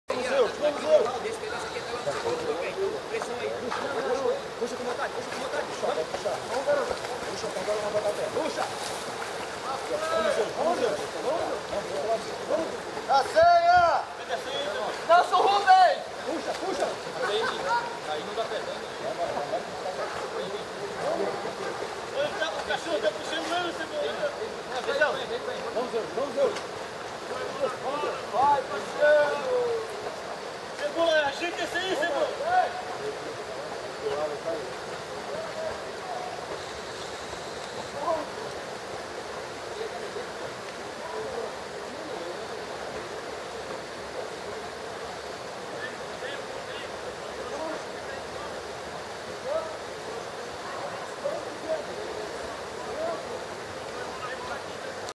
Curso de Cinotecnica da PMSC, Transposição do cabo submerso
Condutor Marcos Paulo Cattoni, Cão Zeus, em instrução a campo durante o curso de cinotecnia da PMSC em 2013, transposição do cabo submerso. Nesta atividade o cão precisa estar adaptado a realizar pois, a tendência dos cão é querer subir no condutor durante a passagem para poder manter-se fora da agua o que implica a travessia para o condutor. Vejam que em nenhum momento Zeus tentou subir e isso é muito importante, os cães tem bastante capacidade de flutuação podendo até auxiliar o condutor no caso de socorro. Todas essas atividades devem ser realizadas no decorrer dos treinamentos da vida do cão policial.